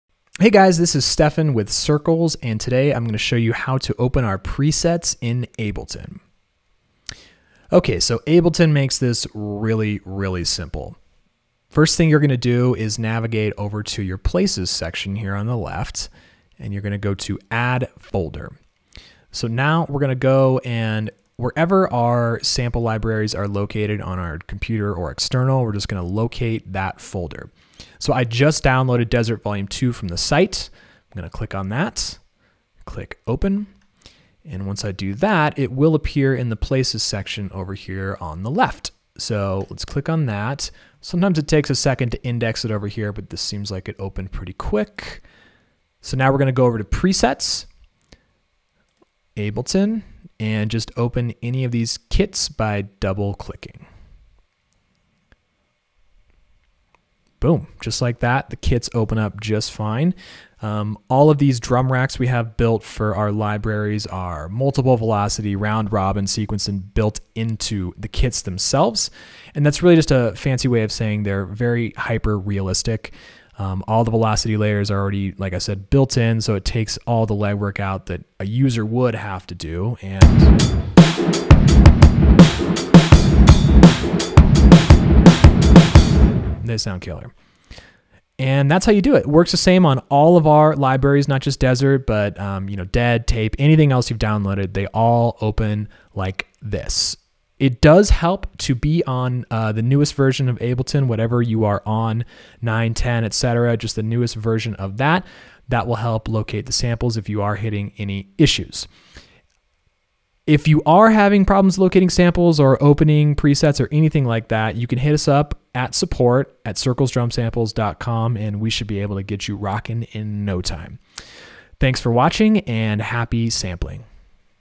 Importing Drum Rack Presets Into Ableton
In this tutorial, we show you how to open our drum rack presets in Ableton.

See Step By Step Instructions Below:

(1)

Simply add the ‘​CIRCLES DEAD VOL 2​’ folder to the ‘places’ section of the sidebar. Open the Live browser and click on "Add Folder..." at the bottom of the ‘places’ section.

(2)

Select the ‘​CIRCLES DEAD VOL 2​’ folder and click "OK”.

(3)

The ​CIRCLES DEAD VOL 2​ drum racks (.adg files) and samples will now be shown in the places section of the Ableton Live browser.

Ableton presets will appear in the ‘PRESETS’ folder. Click on the “PRESETS” folder - Open the “ABLETON” folder to access the Ableton drum racks (.adg file extensions).

NOTE:​ ​If Ableton is not able to locate the files, perform an automatic search of your system.

Use this link to learn how:​ ​goo.gl/zhNJb3

NOTE 2:​ Depending on your version of Ableton, it is possible that you may need to re-save the drum racks after you have performed an automatic search of your system. This can be done by clicking the floppy disk icon at the top right of the drum rack.